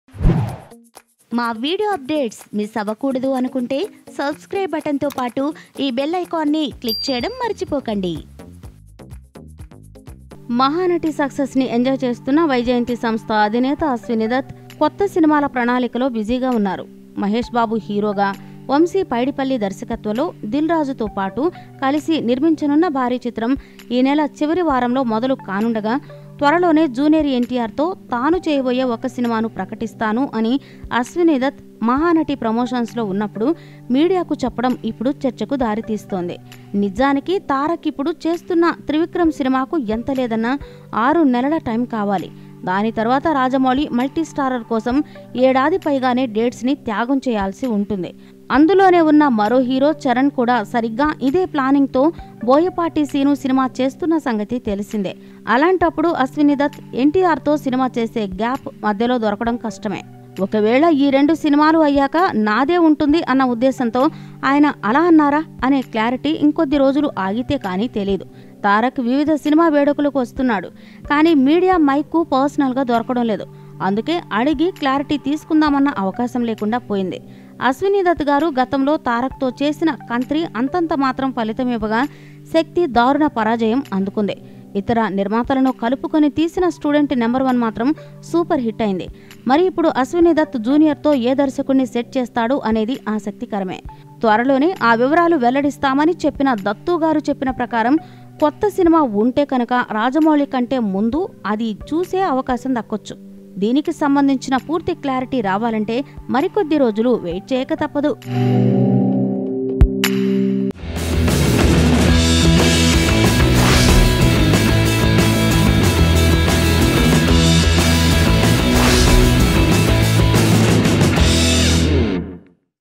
Aswini Dutt Trying his Luck with NTR | Latest Telugu Movie News | JrNTR | Telugu Panda
Tollywood Producer Aswini Dutt / Ashwini Dutt Trying his Luck with Jr NTR. Present, Ashwini Dutt was enjoying the Success of Keerthy Suresh's Mahanati Movie which is produced by his daughters Swapna Dutt, Priyanka Dutt.

Pawan Kalyan's Political Target Will SHOCK You!

Do You Know Why we Should Drink Only Pot Water in Summer?